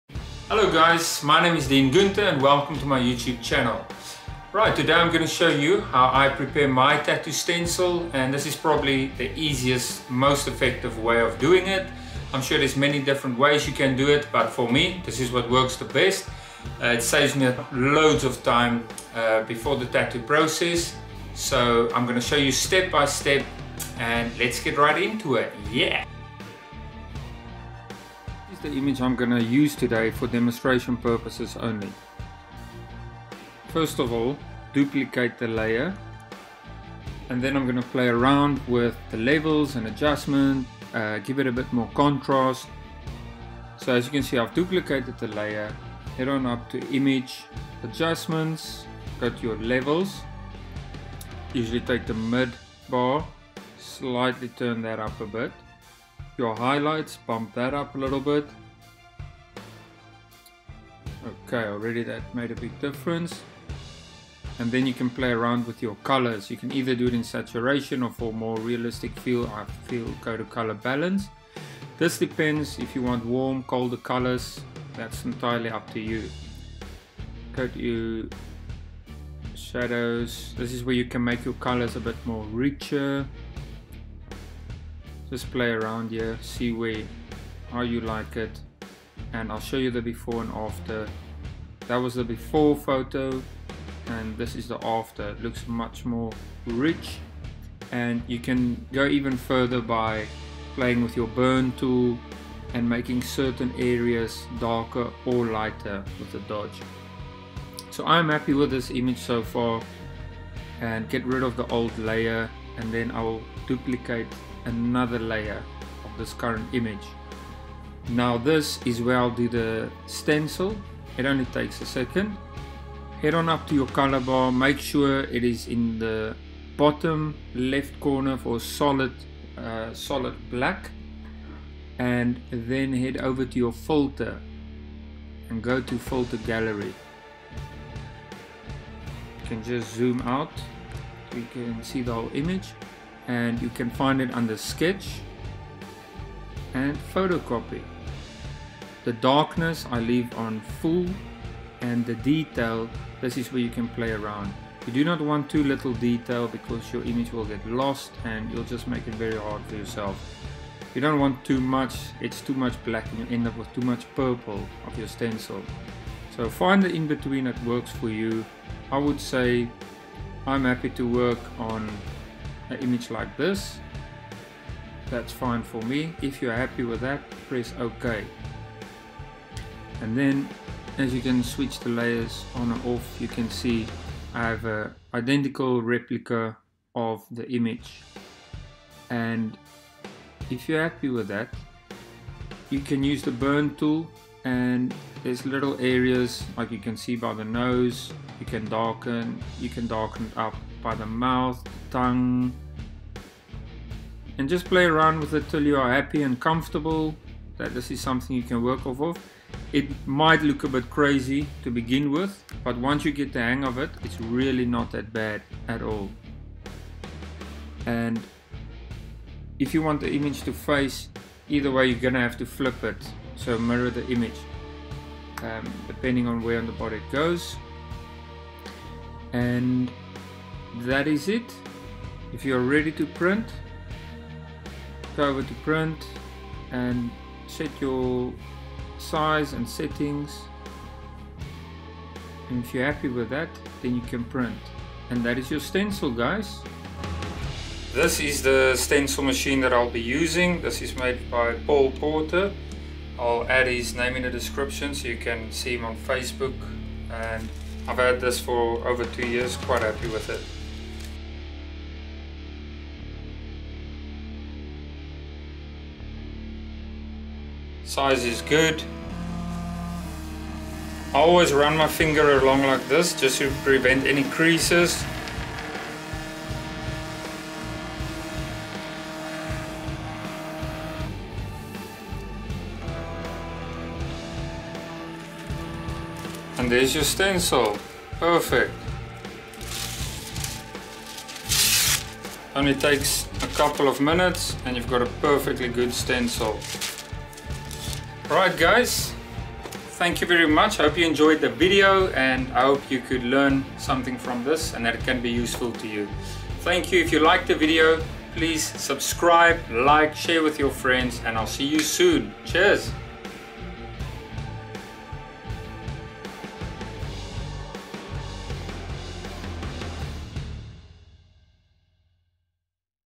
How to create tattoo stencil | Fast and easy | Step by step
This is one of the fastest ways of creating a tattoo stencil using any image. It literally only takes seconds to create.
I use this method to create all my stencils and it saves me hours.

I hope I make things clear and you find my tutorial video helpful.

If you have any questions, please don't hesitate to ask below and I will get back to you.

Stencil Printer I use is made by Paul Porter. You can find him on Facebook through this link.

Facebook: Dean Gunther Tattoo Art

Thank you and happy tattooing!